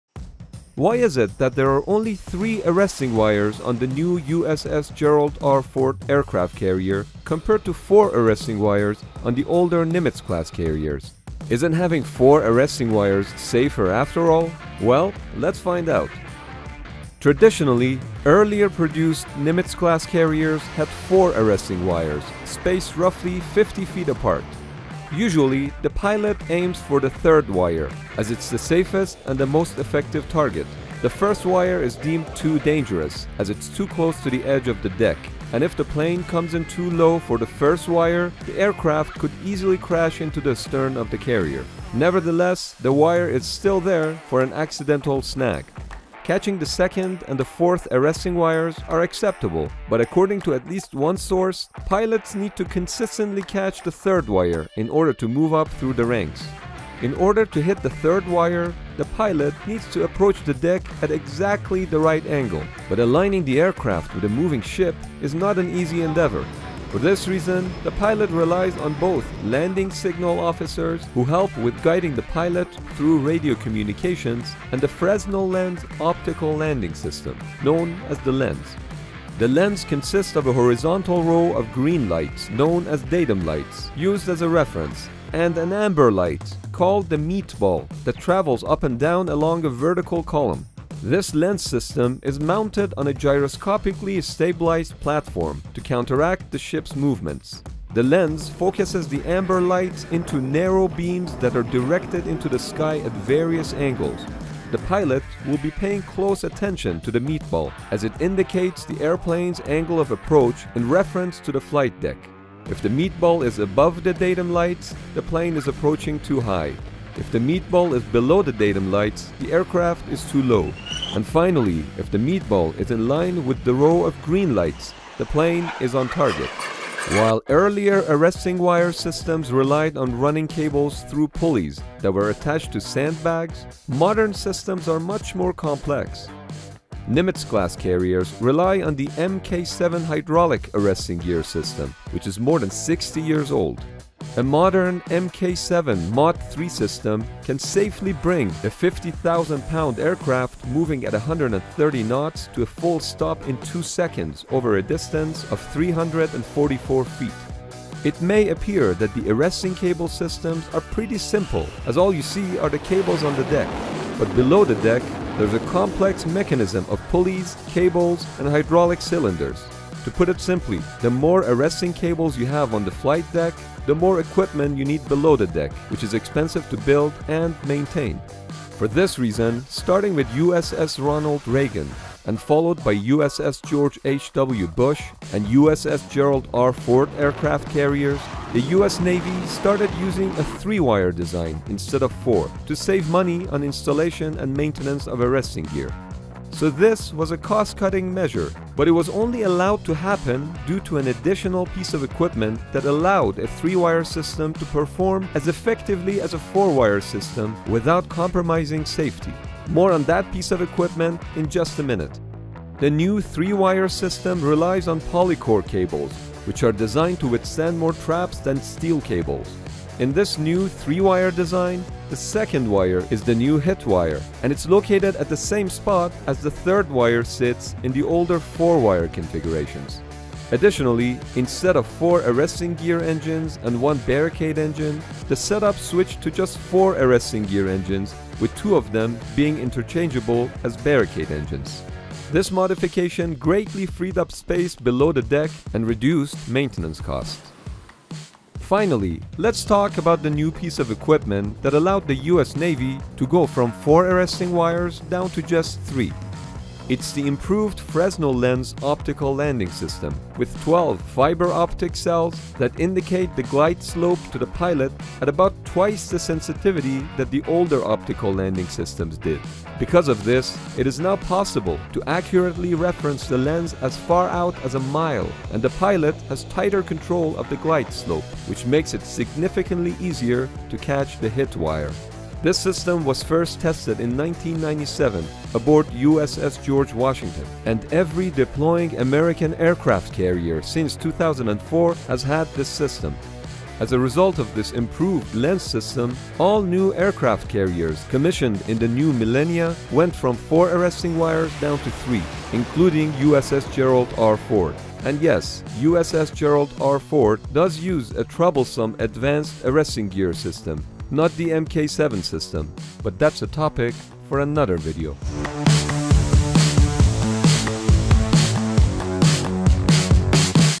Why Aircraft Carriers Lost One Arresting Wire?
We were curious why new aircraft carriers have only three arresting wires instead four on older Nimitz carriers. While the answer may seem obvious, the exact reason for this change is not what you think, or is it?